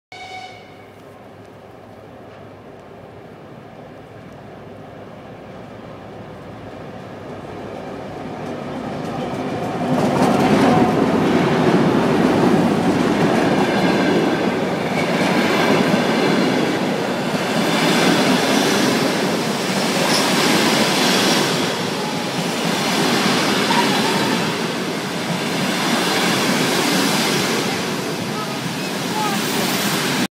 Ukrainian Railways: Moscow-Chernivtsi Train Arriving in Ivano-Frankivsk
The Moscow-Chernivtsi, Ukraine train arrives in Ivano-Frankivsk, Ukraine on 5 October 2011. I boarded this train and rode in 3rd class to Chernivtsi.